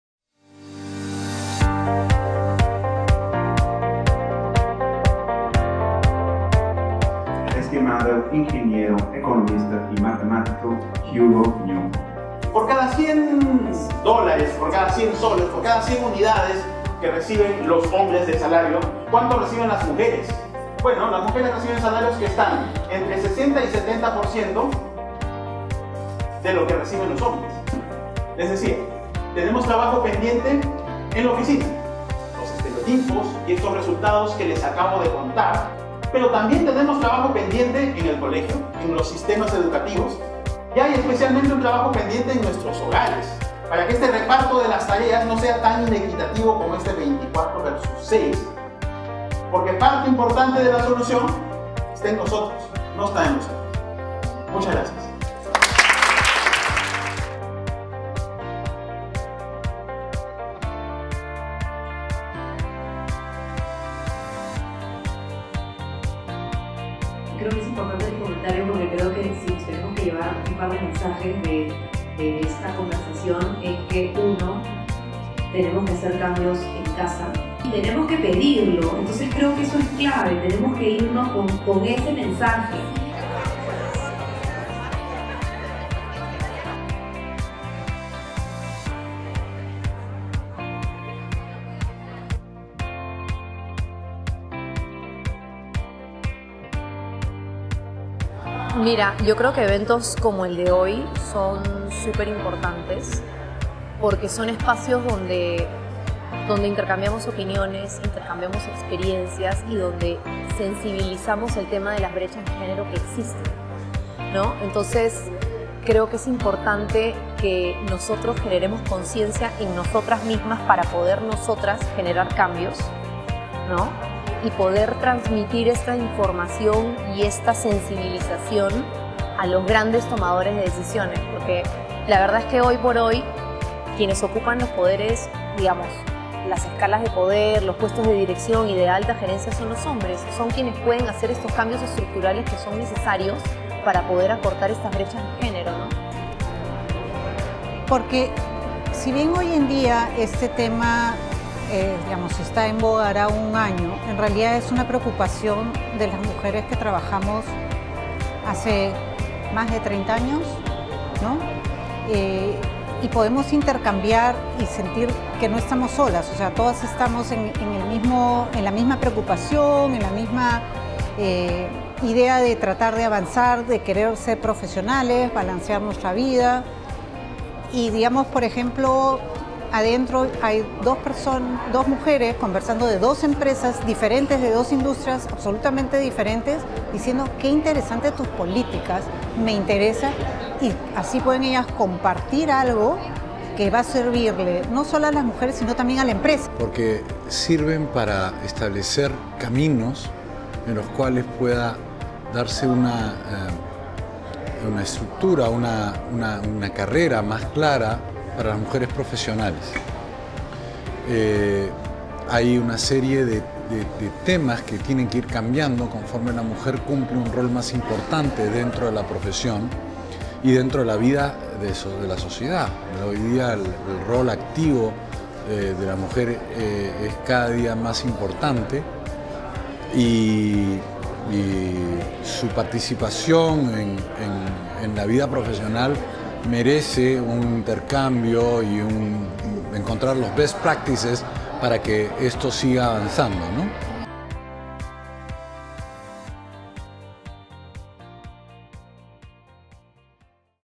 ESOLA | The Legal 500 and Estudio Olaechea Women in Law Summit: Perú - 23.08.2018
Video resumen de lo que fue la primera edición de este evento realizado junto con la reconocida publicación internacional The Legal 500, donde tuvimos la oportunidad de compartir y conocer la experiencia de grandes líderes legales del país en materia de igualdad de género.

Contamos con la participación de Hugo Ñopo, investigador principal de GRADE,  quien presentó sus aportes frente al rol de la mujer en el mercado laboral y legal en el país, así como un destacado panel conformado por Ana Milagros Ferrucci Greco, chief legal officer de Graña y Montero; Silvana Perez, chief compliance officer & legal manager de Komatzu Mitsui Maquinarias; Maria Cecilia Villegas, directora ejecutiva de Prospera; Rossana Natteri, socia del Estudio Olaechea y Tim Girven líder para Latinoamérica de The Legal 500.